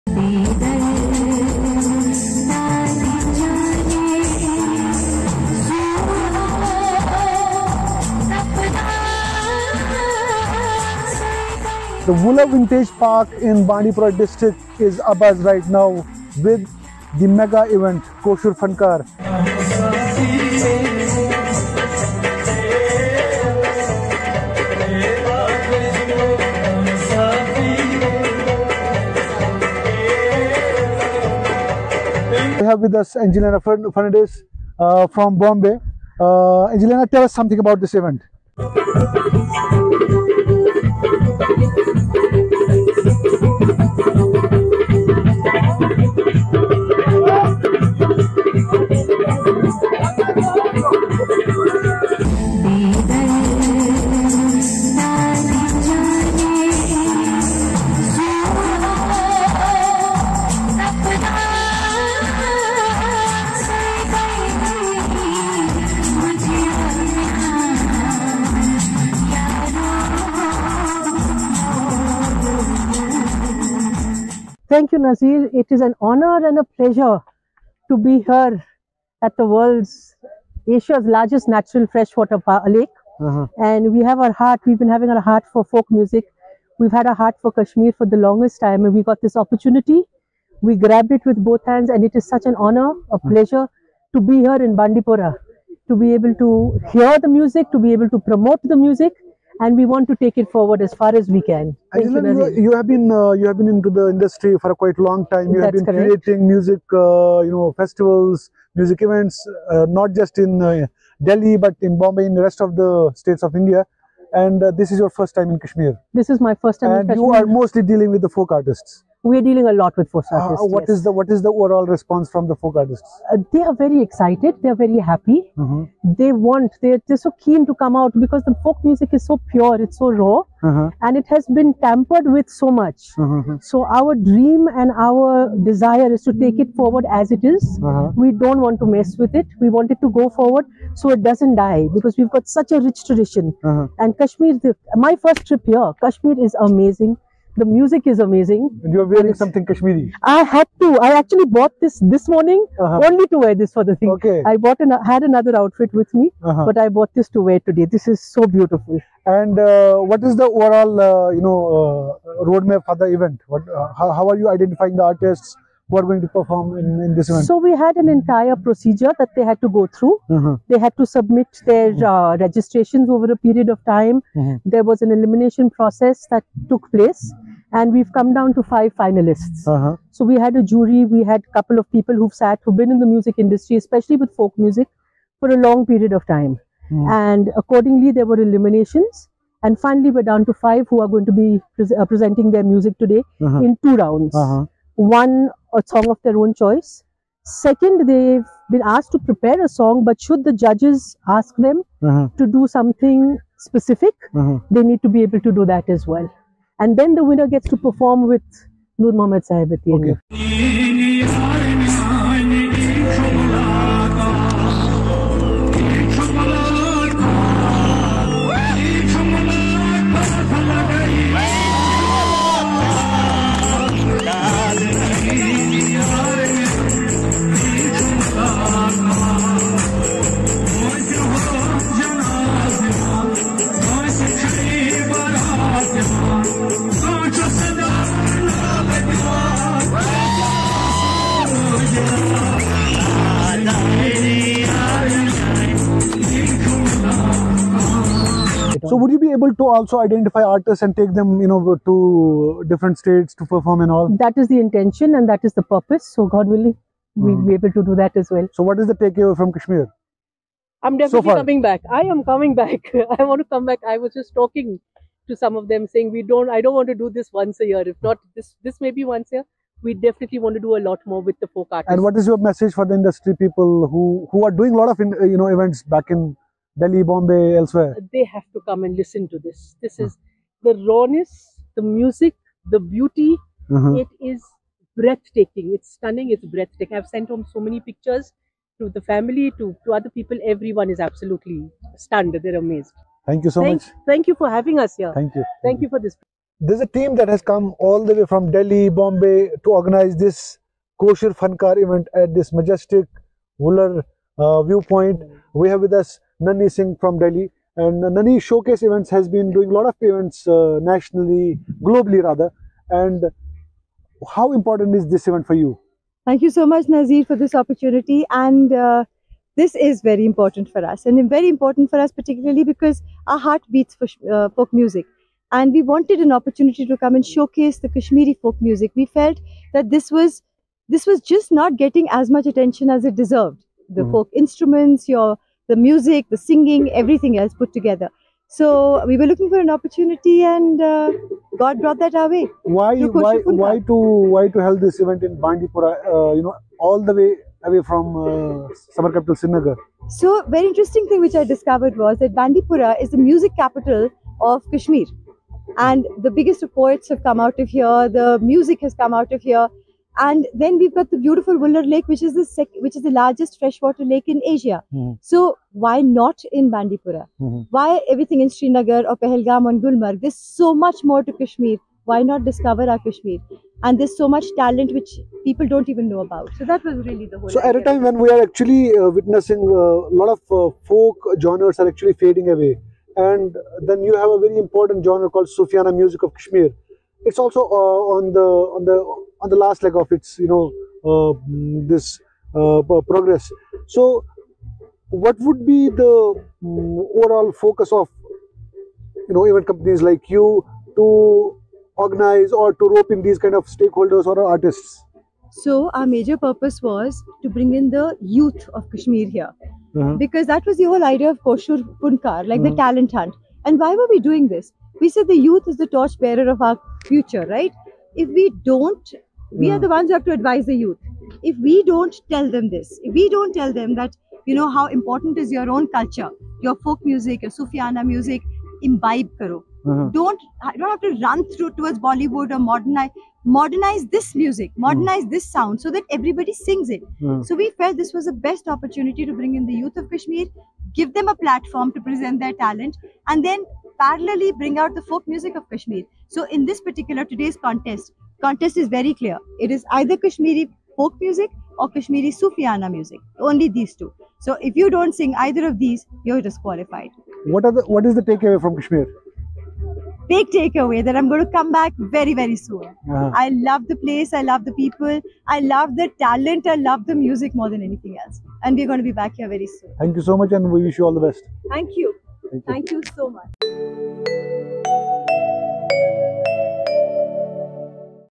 بانڈی پورہ، کشمیر میں 'کوشور فنکار' فیسٹیول میں اژدہام 'Koshur Artifact festival in Bandipur Kashmir
کشمیر میں مقامی ٹیلنٹ کو فروغ دینے کے مقصد سے ایشیا کی سب سے بڑی میٹھے پانی والی وولر جھیل میں ایک میگا ٹیلنٹ ہنٹ کوشور فنکار شروع کیا جا رہا ہے جسے لوگوں کی جانب سے بے حد پسند کیا جا رہا ہے۔ کوشور فنکار کا آغاز کشمیر کے نوجوان ٹیلنٹ کو موسیقی میں آگے لانے کے لیے کیا گیا ہے۔ پدم شری ایوارڈ یافتہ مقامی کشمیری، فیصل علی ڈار، کوشور فنکار کی پہل سے، جموں و کشمیر کے بانڈی پورہ ضلع کے وولر لیک وینٹیج پارک میں منعقد ہونے والا یہ اپنی نوعیت کا ایک ٹیلنٹ ہنٹ میوزک فیسٹیول ہے۔ اسے علی اسپورٹس اکیڈمی (اے ایس اے) کے زیراہتمام منعقد کیا جا رہا ہے، جسے خود پدم شری فیصل علی ڈار نے قائم کیا تھا، جس نے کشمیر کی وادی میں کھیلوں کی ثقافت کو فروغ دینے میں کامیابی سے حصہ لیا ہے اور کشمیر کے نوجوانوں سے چیمپئنز تیار کیے ہیں۔ اے ایس اے ، شوکیس ایونٹس ، دی ایگل کے ساتھ، اب کشمیر کے موسیقی کے باصلاحیت نوجوانوں کو آگے بڑھانے اور انہیں ہندوستانی ہنر کے مرکزی دھارے میں لانے اور کشمیری لوک اور روایتی موسیقی کو فروغ دینے کے لیے کام کر رہا ہے۔